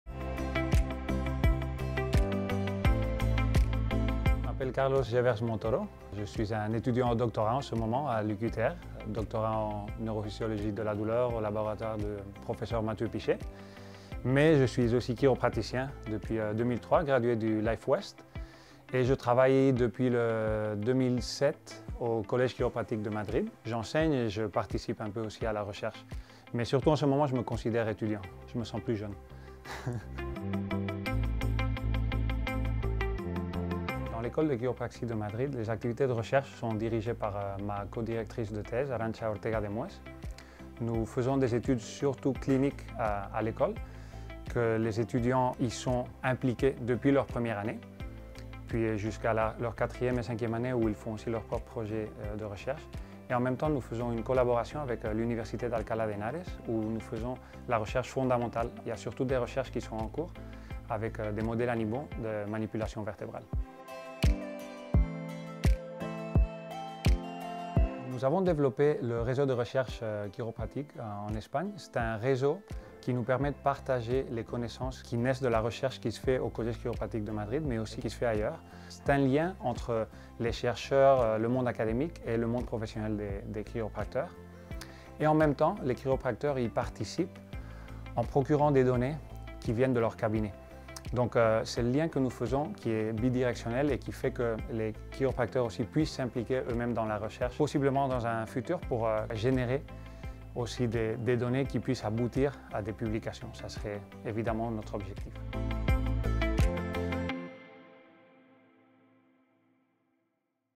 Interview de Carlos Gevers Montoro pour l'IFEC
Interview réalisée pour l'IFEC, école de chiropraxie.

Researching mechanisms of hypoalgesia involved in pain inhibition during spinal manipulation. In particular peripheral mechanisms linked to neuroinflammation, mechanisms of central sensitization and oscillatory brain mechanisms.